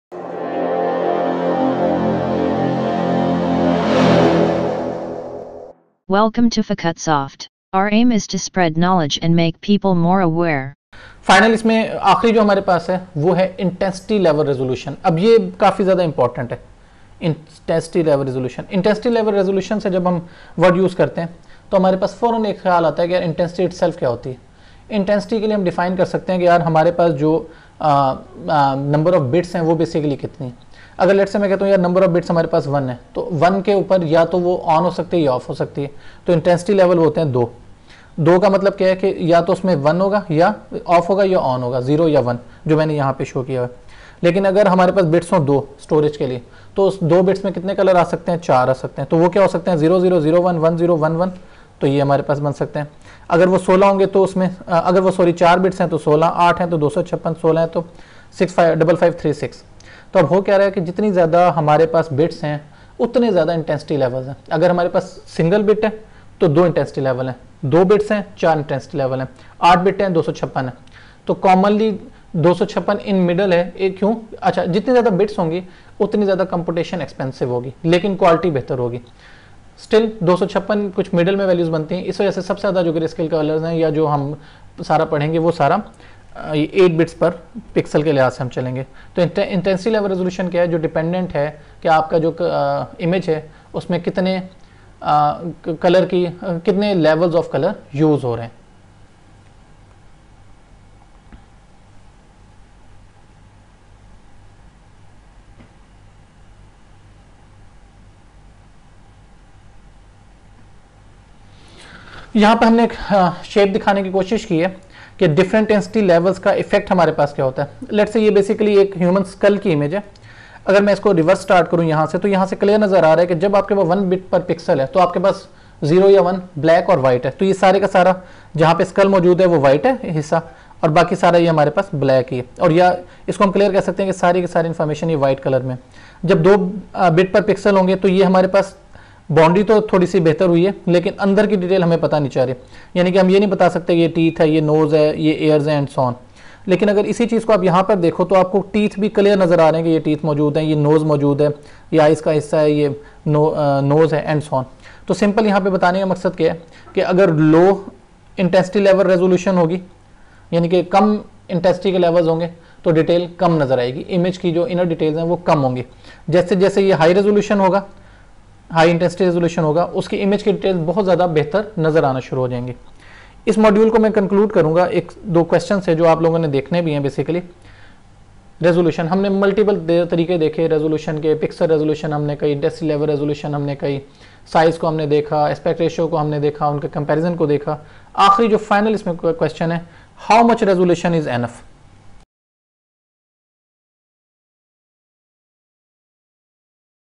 Digital Image Processing (19) || Intensity Level Resolution || Urdu || Hindi
Digital Image Processing ! What Is Digital Image Processing? Intensity Level Resolution. Digital Image Processing for Beginners and students
by Dr Usman Ghani Khan